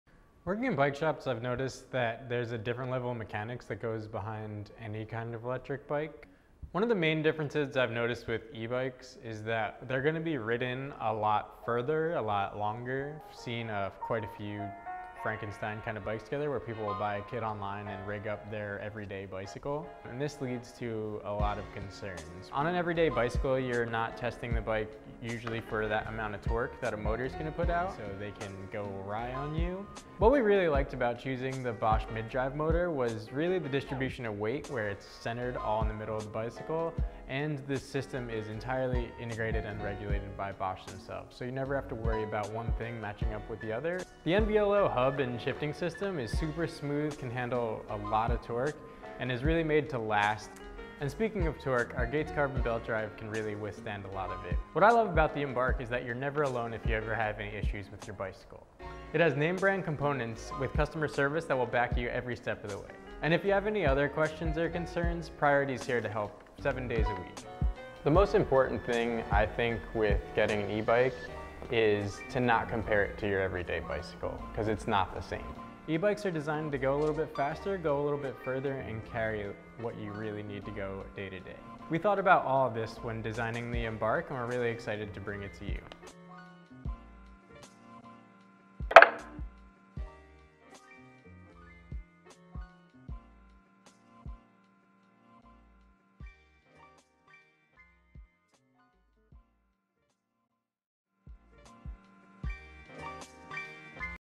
PRIORITY BICYCLES: Engineering The Embark
How it is important to have components and a frame design that is built to take on the torque, weight, stopping demands, and accessories of an eBike.